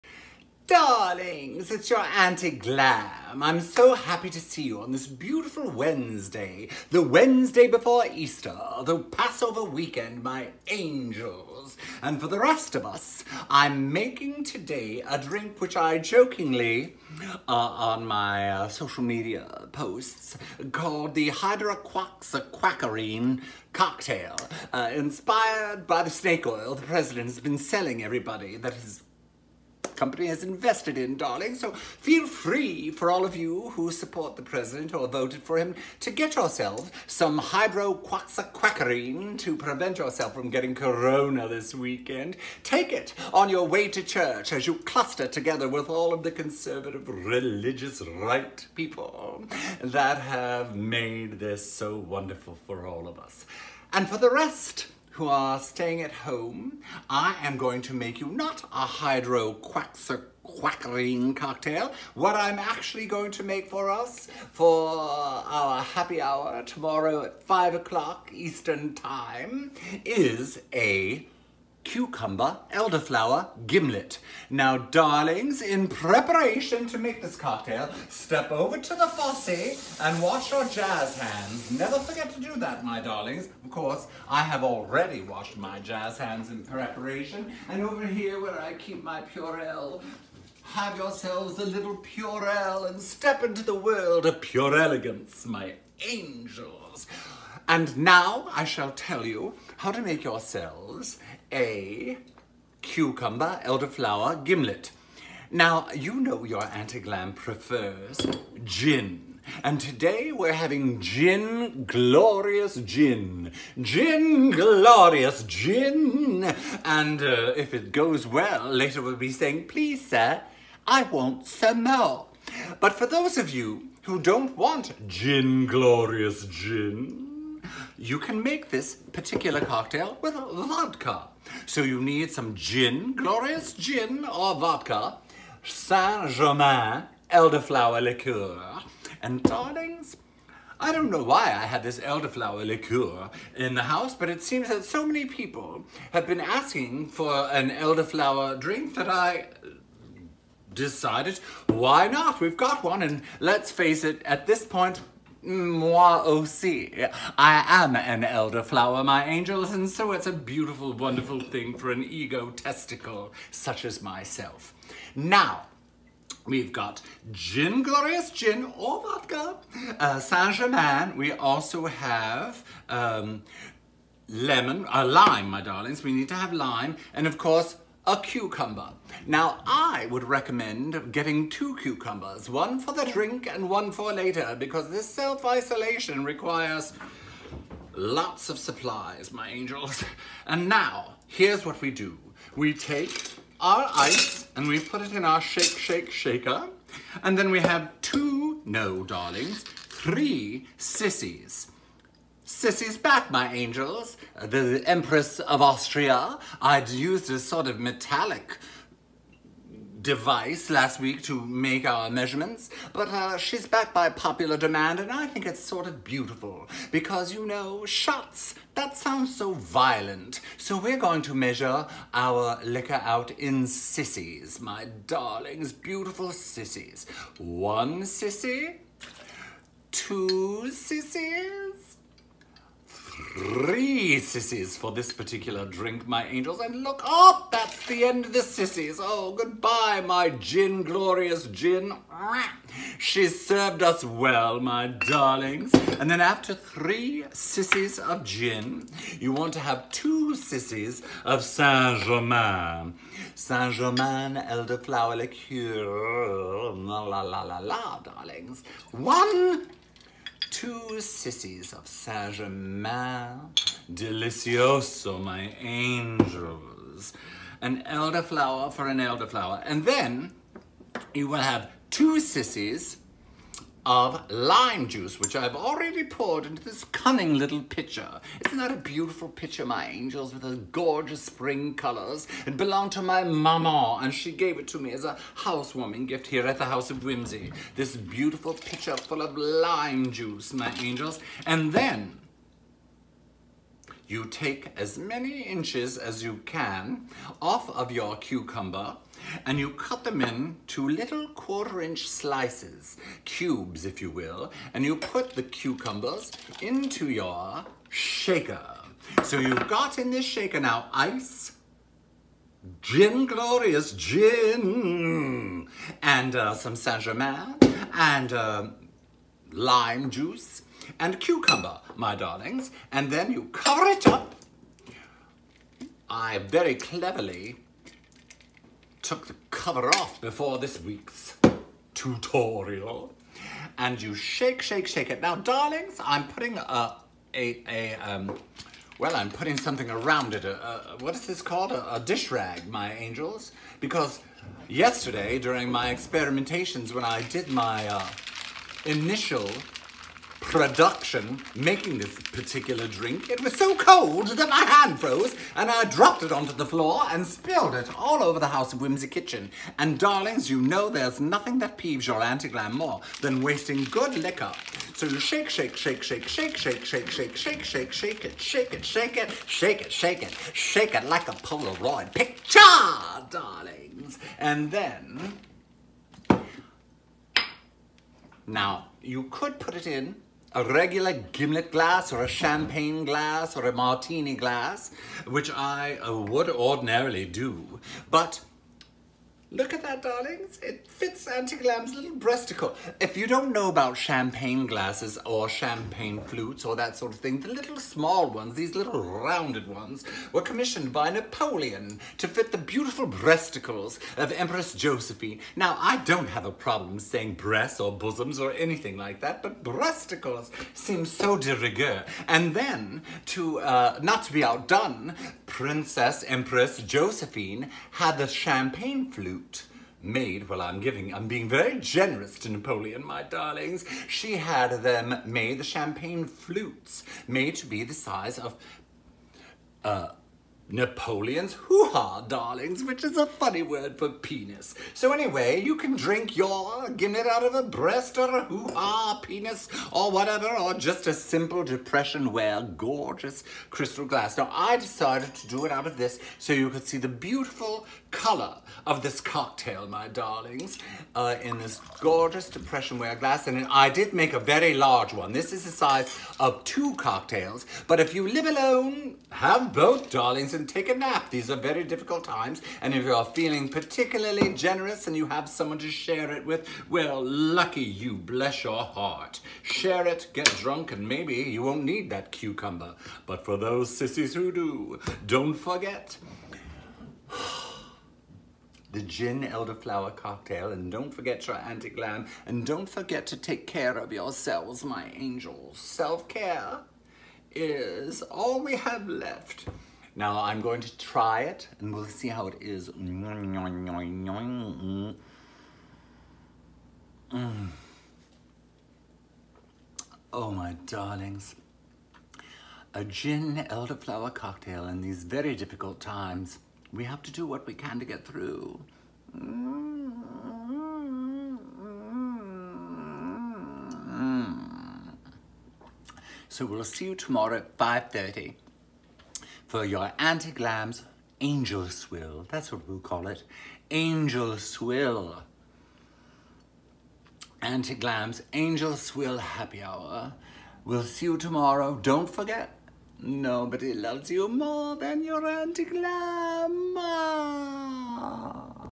Your Auntie Glam’s Hydro-Quacksy-Sequin Cocktail Tutorial
IT’s HAPPY HOUR!!! .
Here’s Your Auntie Glam’s weekly quarantini cocktail tutorial. Shake your own and join us tomorrow (Thursday) at 5pm ET at justinvivianbond.com for our weekly live-screamed, social-distanced performance rrrrritual! .